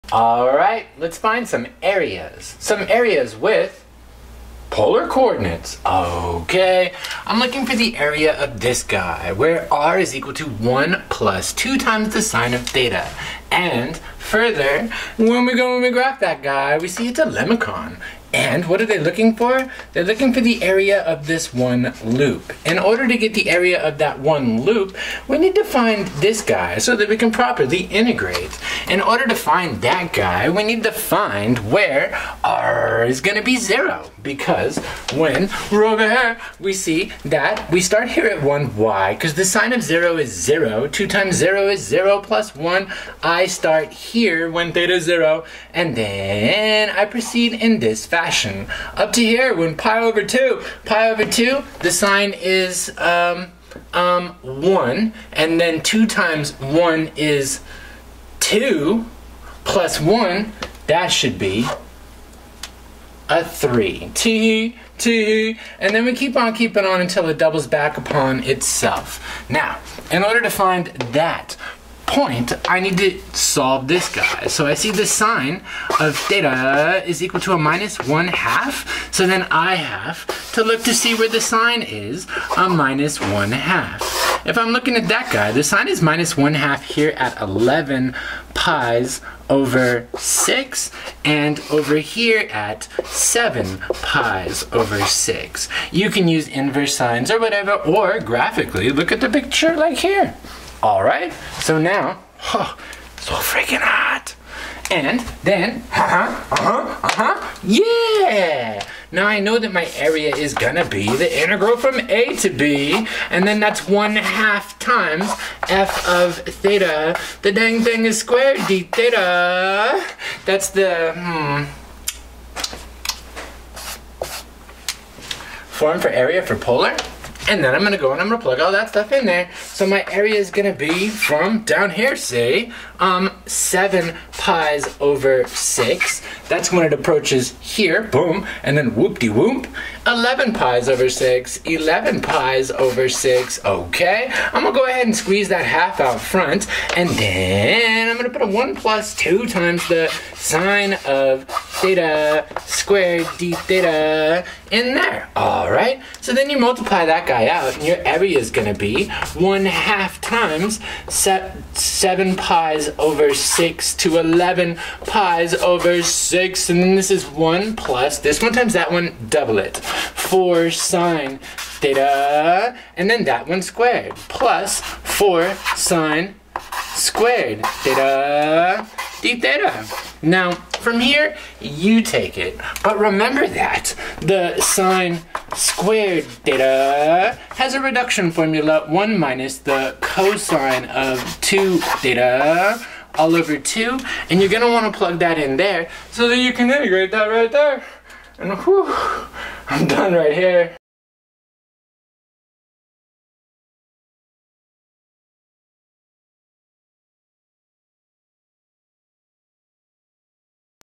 Find the Area Using Polar Coordinates for the inner loop of r=1+2sinx
This is the third video in a series of five for calculus of polar curves.